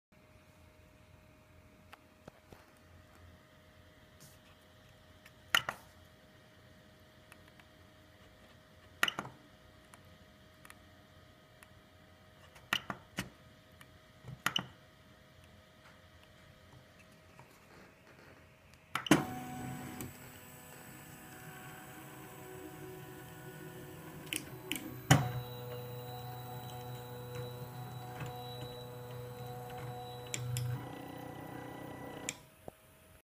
It's Alive! MP3DP in motion.
Auto homing the MP3DP (Milled and Printed 3D Printer) for the first time, second if you count the failed attempt.  Now that the build and wiring are complete, I can start the calibration phase.

Go here to see the Designer's site, get the plans and build your Own!